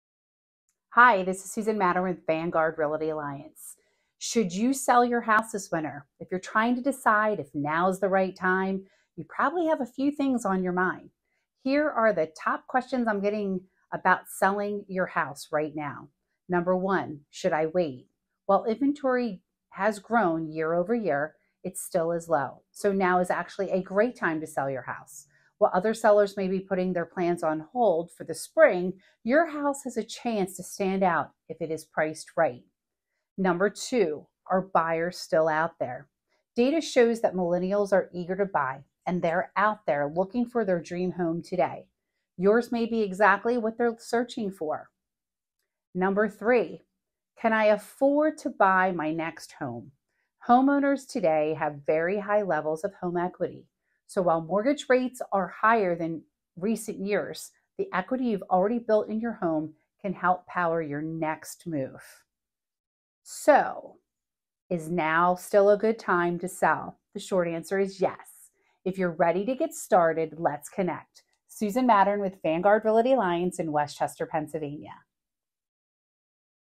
Should you sell your house in the winter?
In this video, we're going to discuss whether or not you should sell your home in the winter.

 Selling your home in the winter can be a lot of work, but it can also be a great time to buy. We'll discuss the pros and cons of selling in the winter, and give you a chance to ask us any questions you may have. After watching this video, you'll be able to make the best decision for your situation!
Why selling your house in the winter is better than you think…